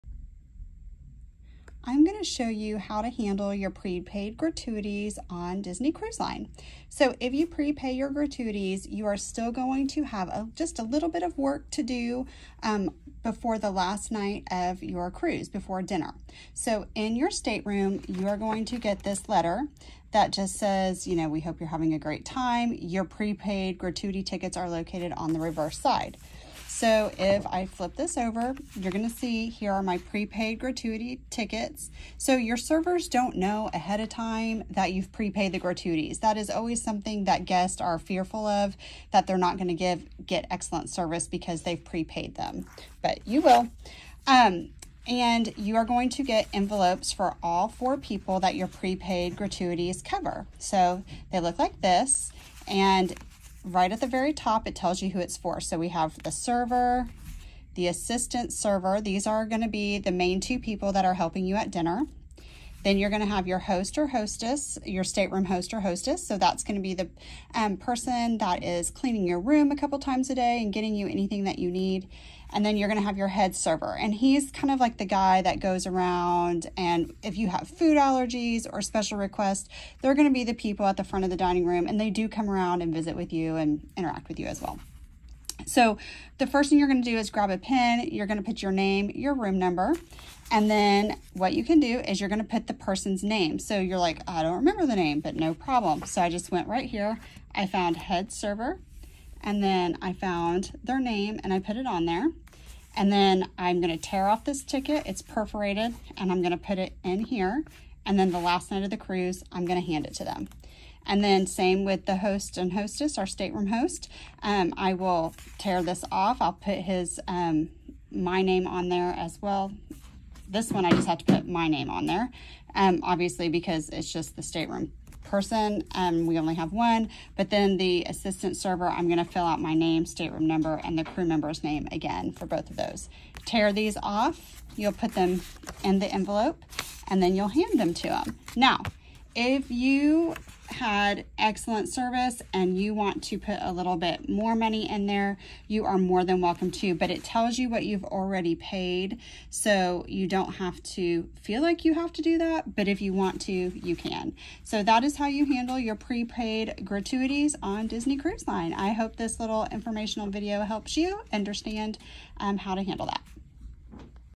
Prepaying your Gratuities on Disney Cruise Line
This video walks you through prepping your prepaid gratuities on Disney Cruise Line.